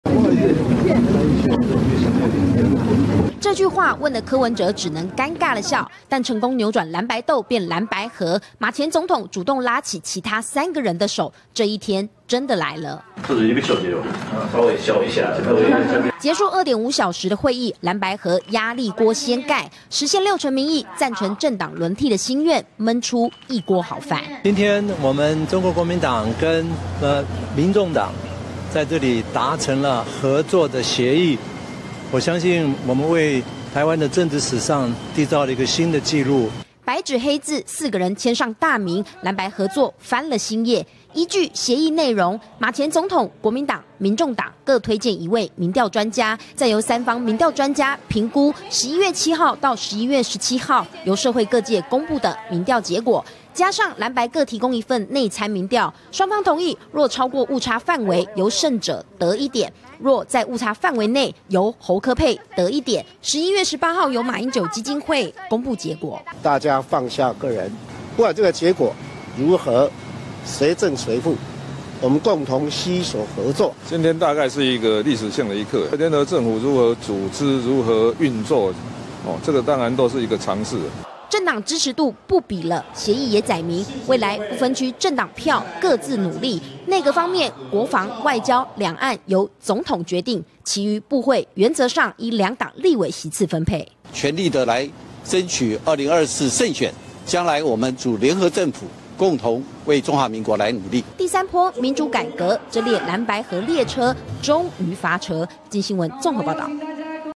侯柯訂婚了！4方會談達成6點共識 週六見真章｜鏡新聞
藍白合終於大躍進，今天（11/15）上午歷經2.5小時藍白政黨二次協商，決定由藍白各推薦一位民調統計專家，檢視評估11/7至11/17，社會各界公布的民調結果，以及國民黨、民眾黨各提供一份內參民調的結果，特別的是，雙方同意若超過統計誤差，由勝者得一點，若在統計誤差範圍內，是由侯柯配得一點，究竟是侯柯配還是柯侯配，本週六上午由馬英九基金會公布。